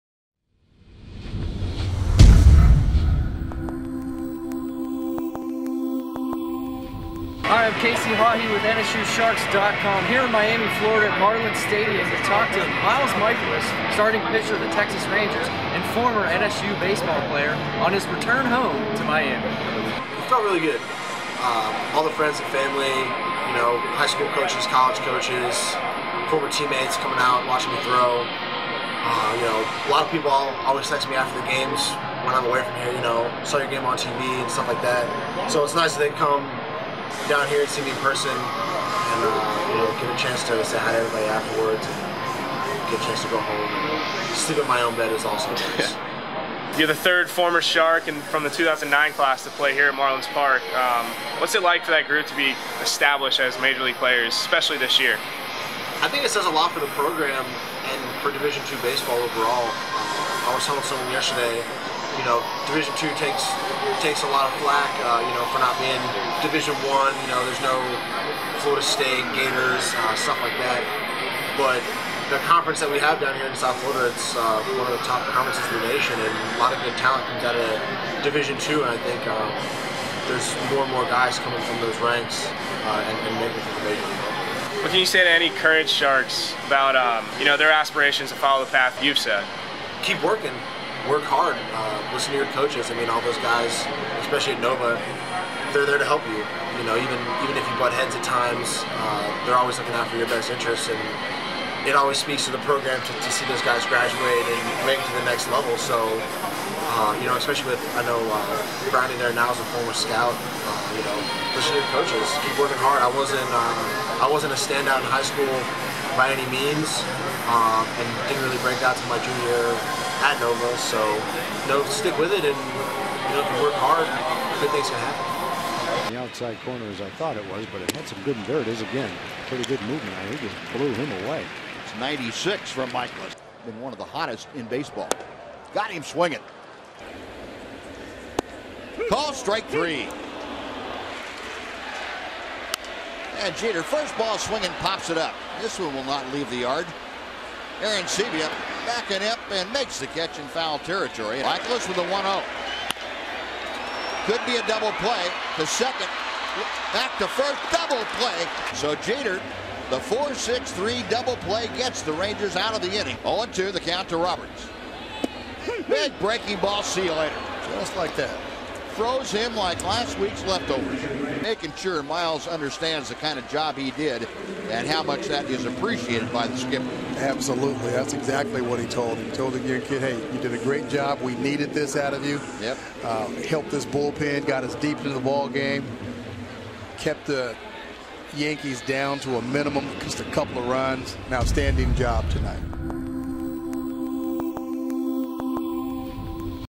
Miles Mikolas Returns Home
Interview with Miles Mikolas (Texas Rangers), NSU Alumni, on his return home to Miami.

Directed/Edited/Filmed by:
KC HAHEY (2014)